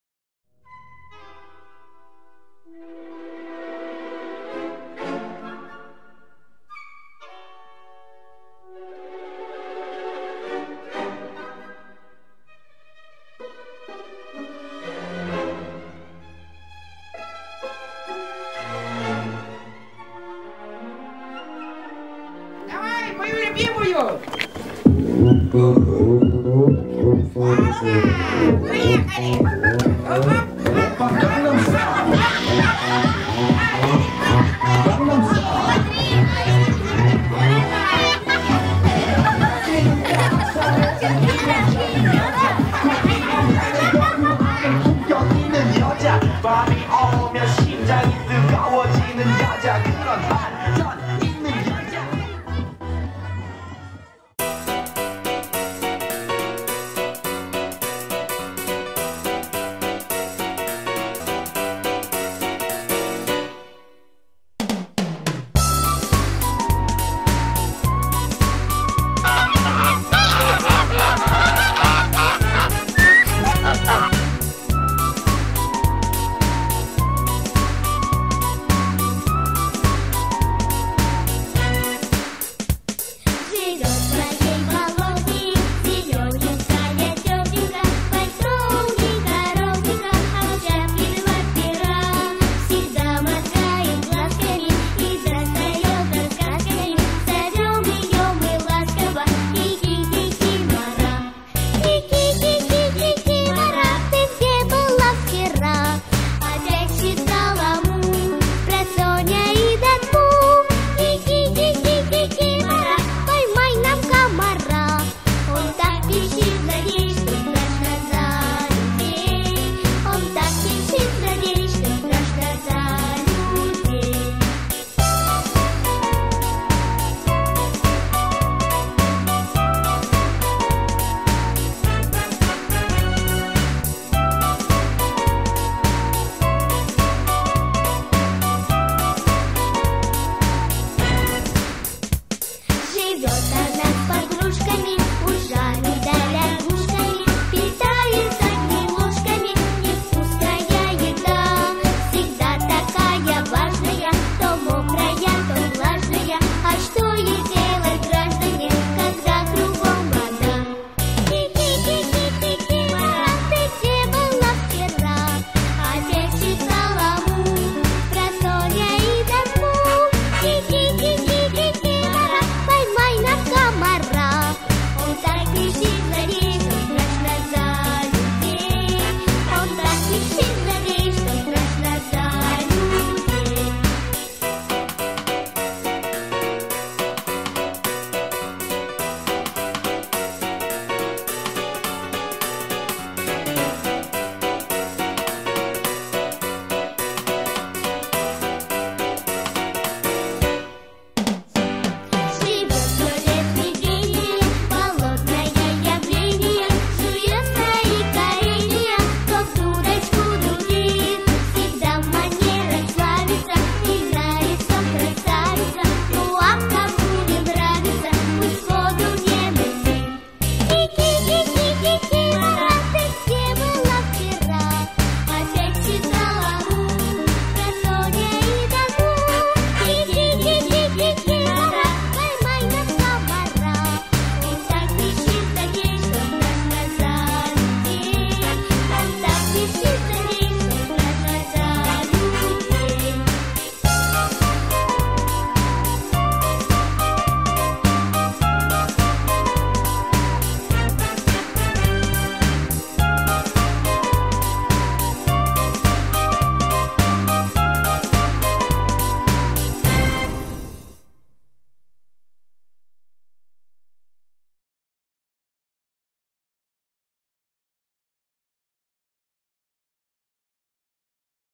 СТРАСТИ У КИКИМОРЫ в Ботово!
В гостях у русской сказки! А Кикимора не простая, а КОВАРНАЯ!!! Дети выполняли сложные, но в то же время веселые и интересные задания! А еще, у Кикиморы есть свой собственный зоопарк! 1 класс 30 школа съемка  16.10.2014